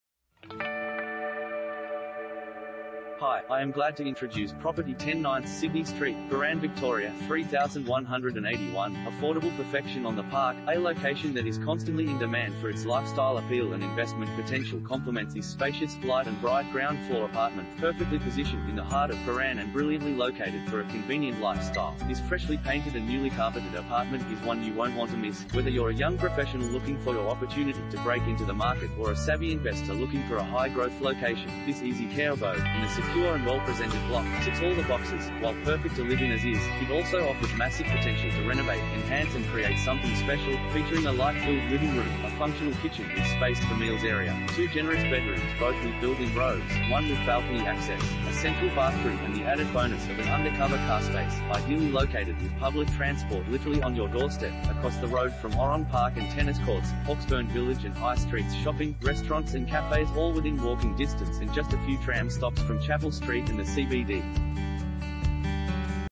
10/9 Sydney Street, Prahran VIC 3181 | AuPropertyReport.Com
10/9 Sydney Street, Prahran VIC 3181
Property Type: apartment
Bedrooms: 2
Bathrooms: 2
Car Space: 1
Affordable perfection on the park